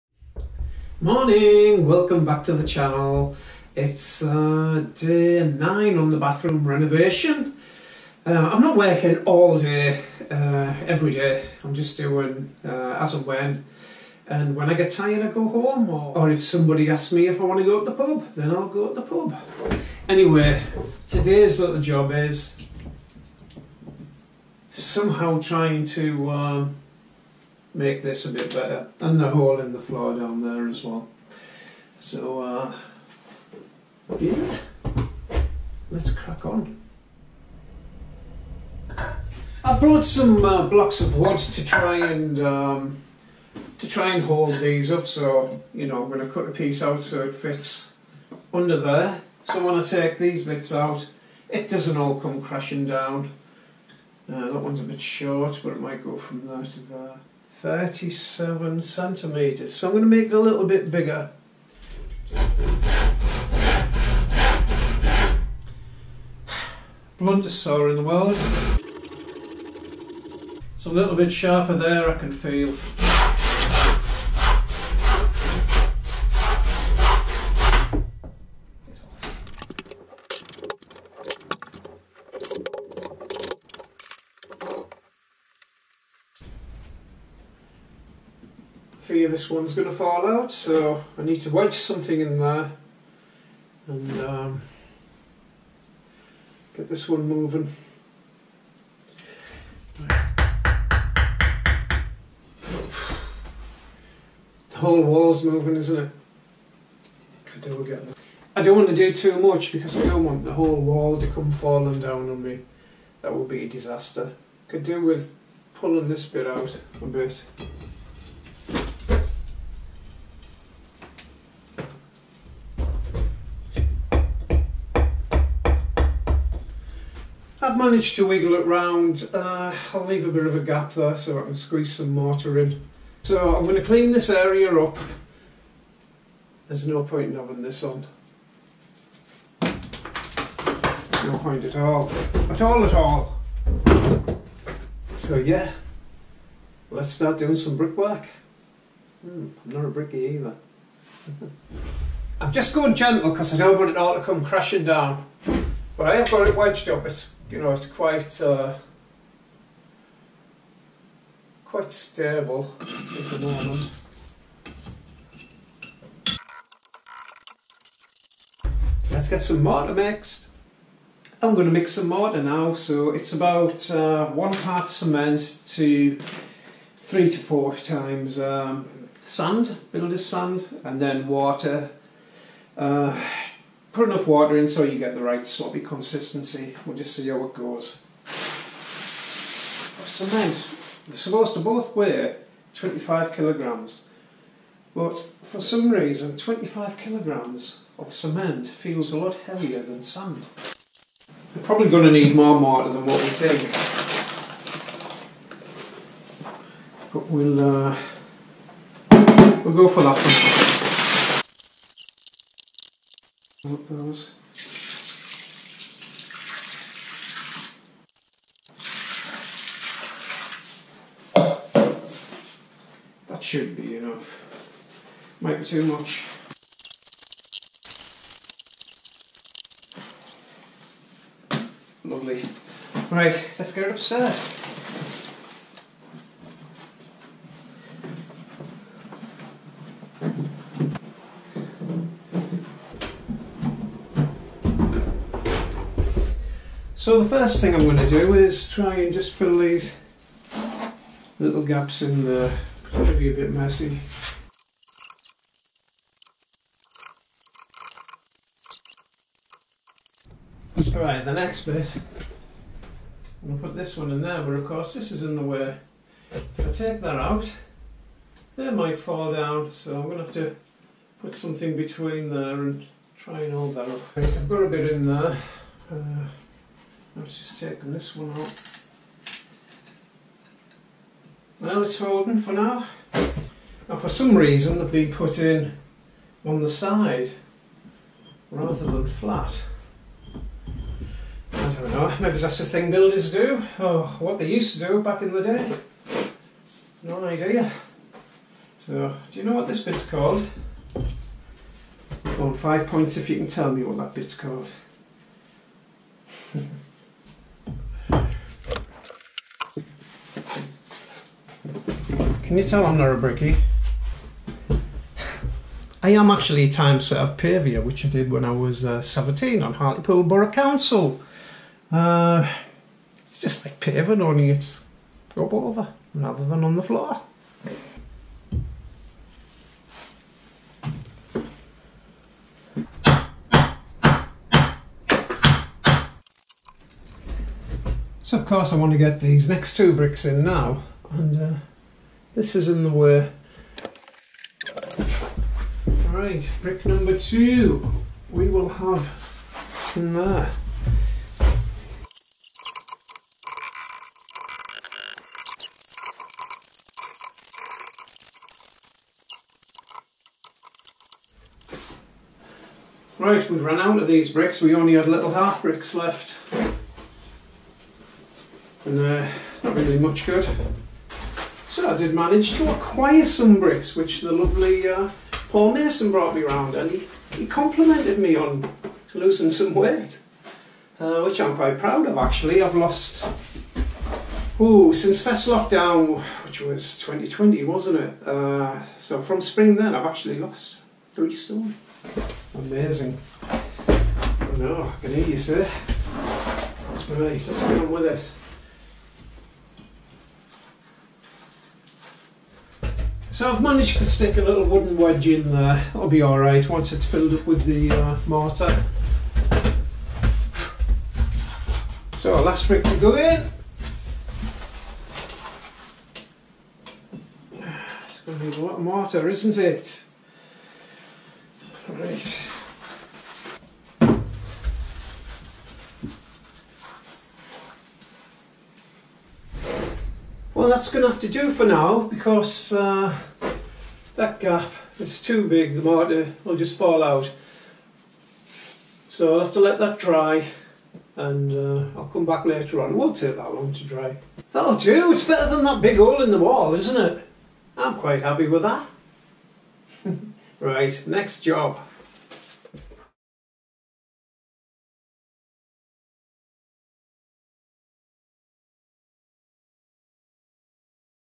How to be a Brick layer. Bathroom renovation Part 9
Today I had to repair a big hole in the wall, before we can plasterboard and tile it.
I'm not a Brickie by any stretch of the imagination, but how hard can it be ;)

Extra bricks supplied by the lovely Paul Mason.